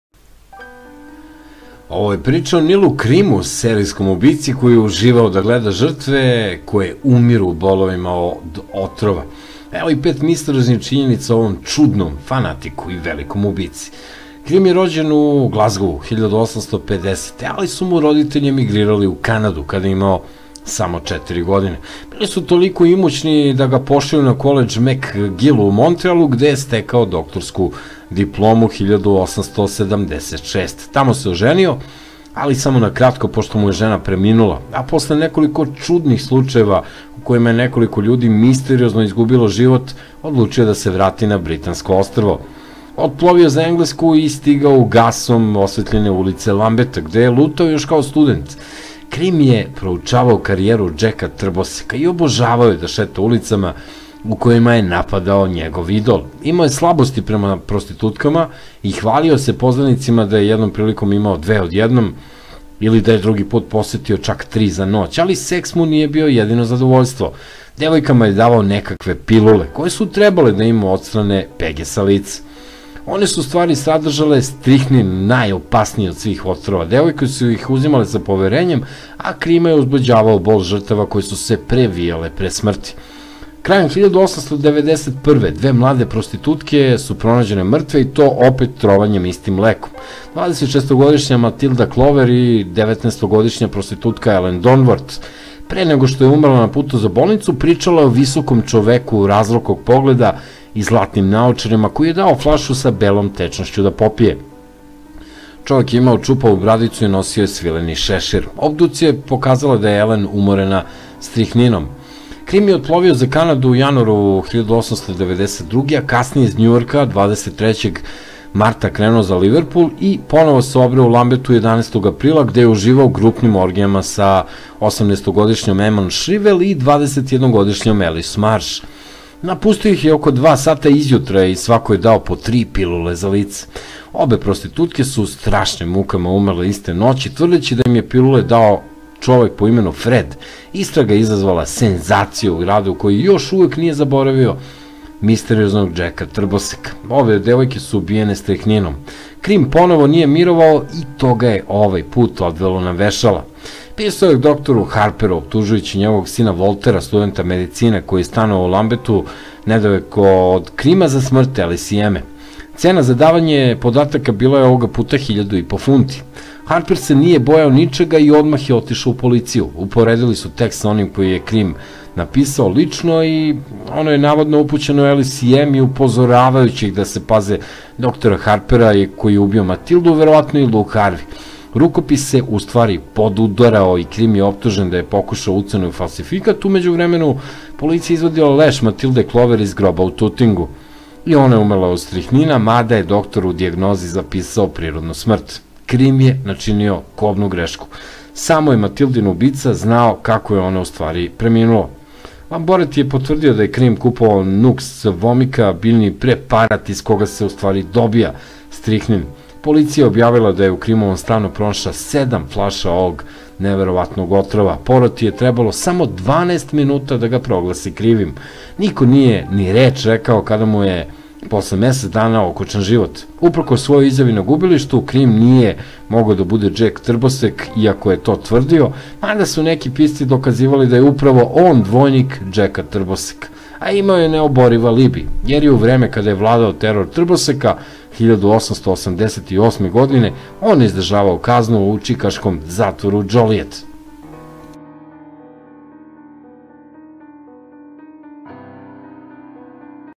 Psihopata Nil Krim
Sredinom 19. veka doktor Nil Krim je masovno ubijao svoje pacijente tako što bi ih otrovao, a onda uživao gledajući ih kako umiru previjajući se u bolovima. Idol mu je bio Džek Trbosek.